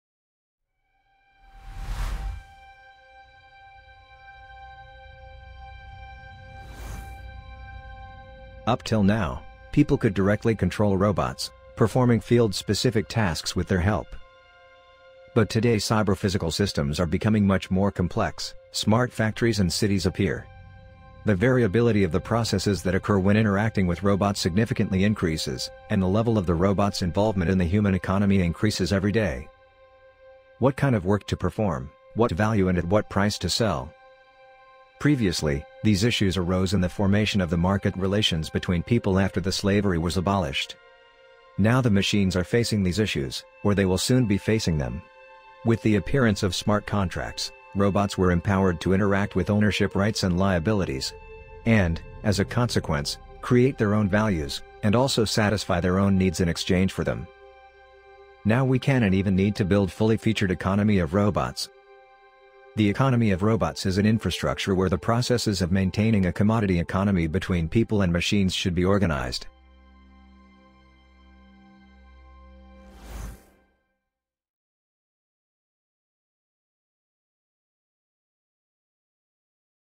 The economy of robots: what is it?
We believe the market mechanism makes a network more sustainable than any other approach in the context of arranging a common work of cyber-physical systems (CPS). The network managed by the market mechanism is capable of self-organization and adaptation to changes over time. Furthermore, the market mechanism requires CPS to be represented as an economic agent what fully responds and solves the issue of representing CPS as a service.

In our vision, Robonomics network is a network of CPS working as a service and based on the market mechanisms.